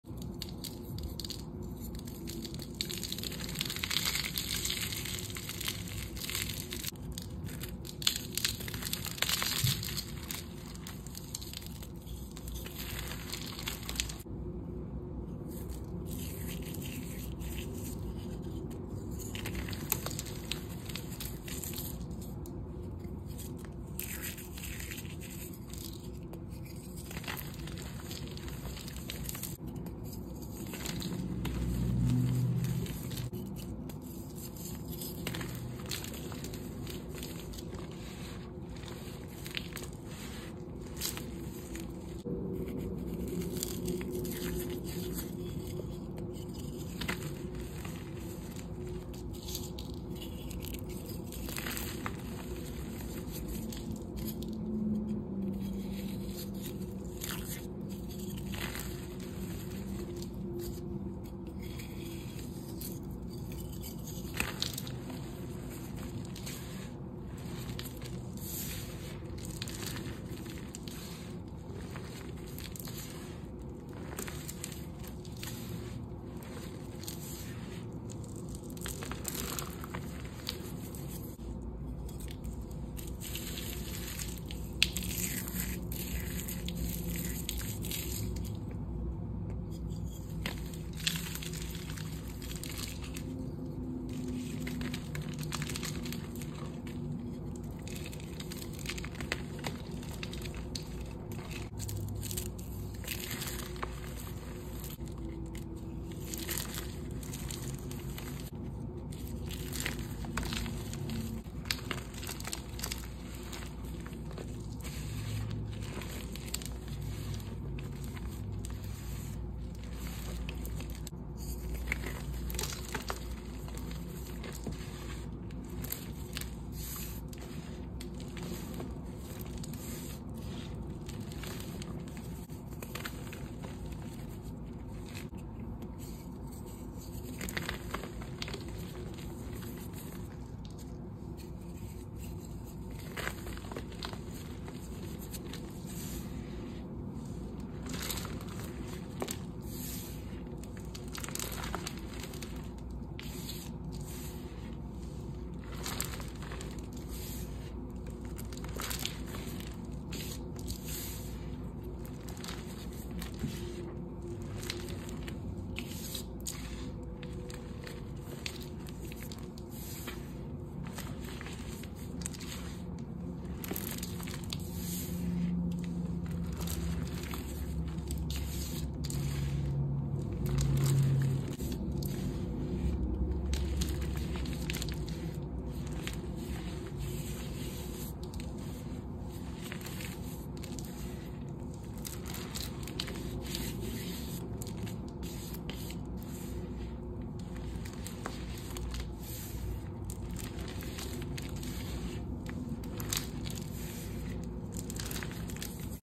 Chalk. By Raindear.ASMR 🍬| Flaky Crust | Soft 👗| No Edits or Repost on any platform.
I ordered some reform powder from Raindear.Asmr  and this powder is literally the creamiest powder I ever felt . It is incredibly soft and makes the best flaky crust ! I will definitely be ordering more so I can make bigger crushes !

🍬👗💜No edits or reposts on any platform